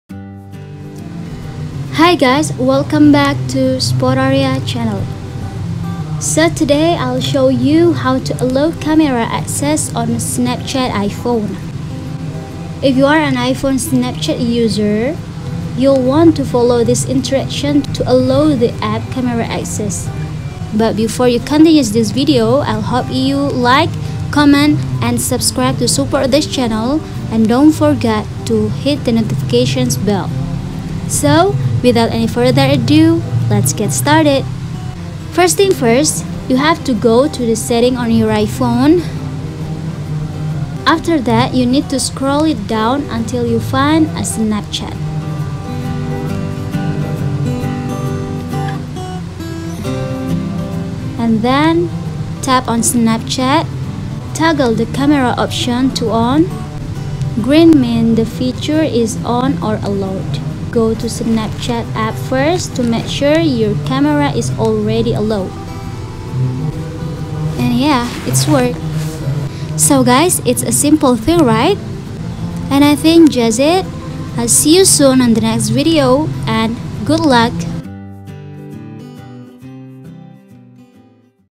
How To Allow Camera Access On Snapchat Iphone
Hello guys, in this video I'll show you how to allow camera access on snapchat iphone.

In order to use the iPhone camera in the Snapchat application, you must allow camera access  to the iPhone settings.

So please watch this video to find out the steps to allow camera access on snapchat iphone.